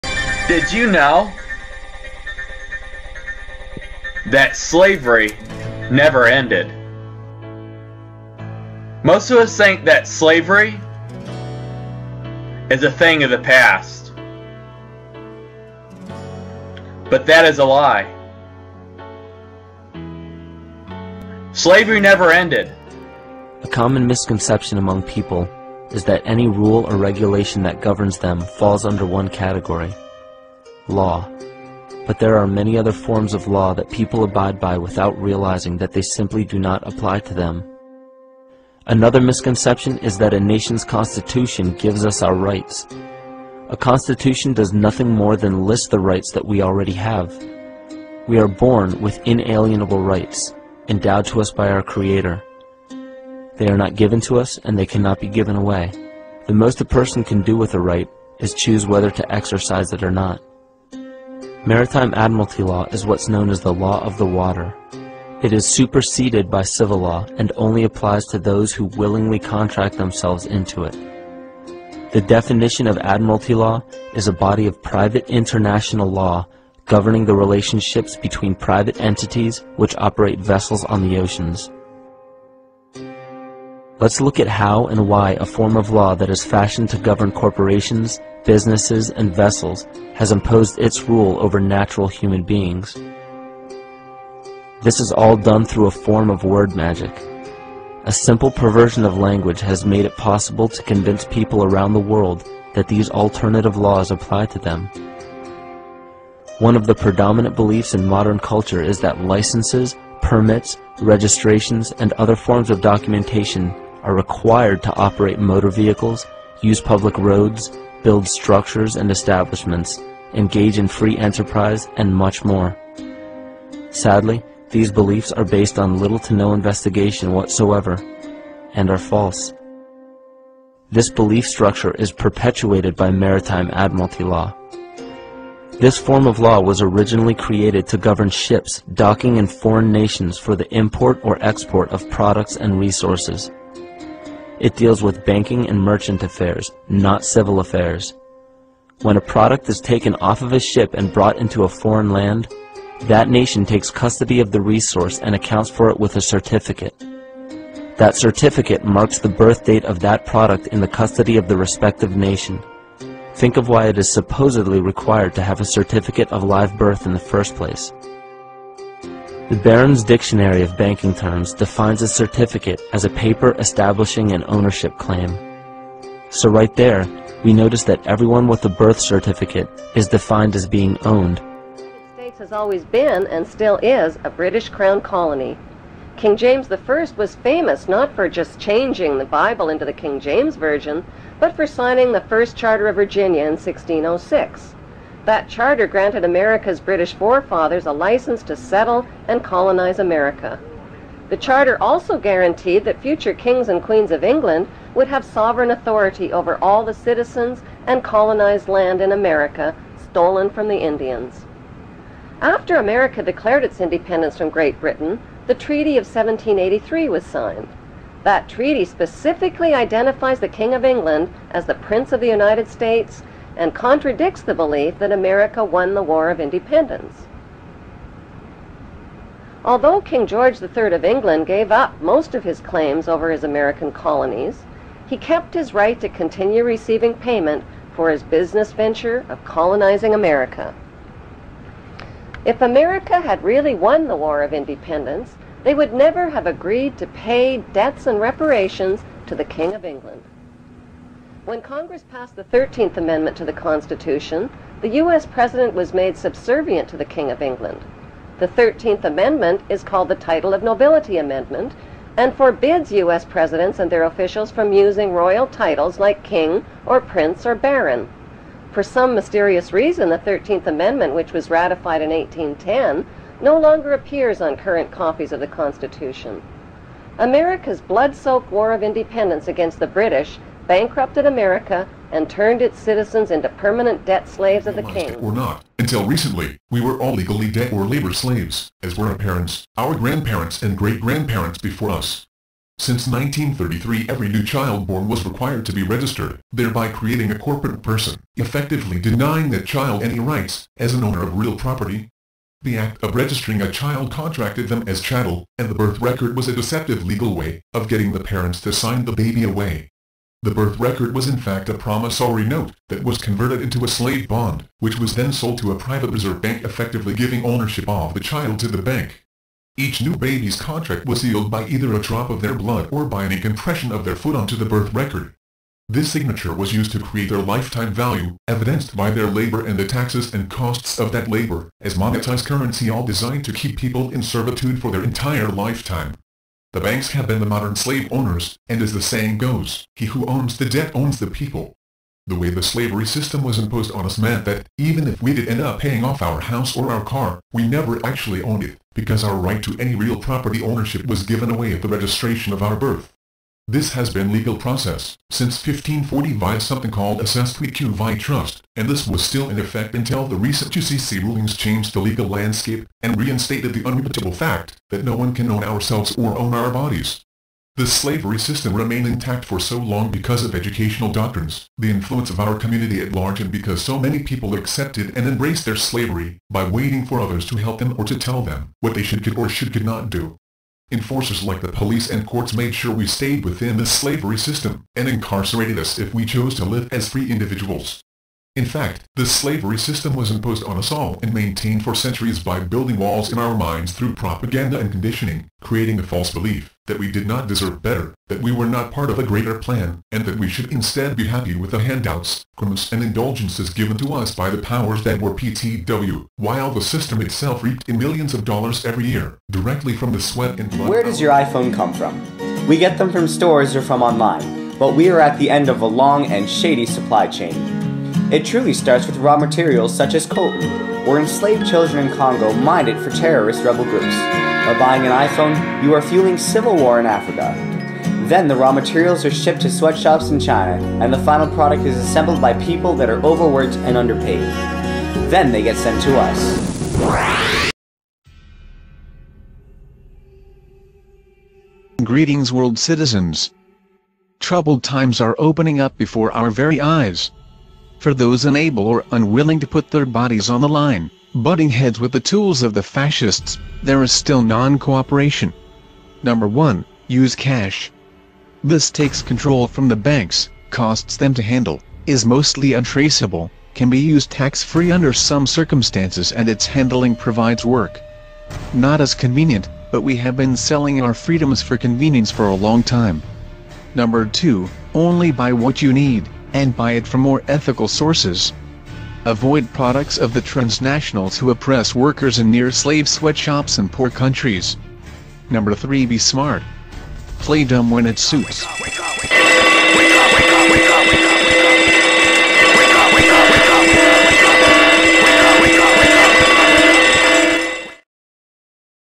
MODERN SLAVES AND THE SLAVING ENSLAVERS WHO ENSLAVE YOU
"SLAVE CAMP AMERICA..." MODERN DAY SLAVER IS REAL! This video provides insight as to the modern origins of present-day slavery. Are you a slave and don't even know it? What this video and find out. YOU WANT TO KNOW!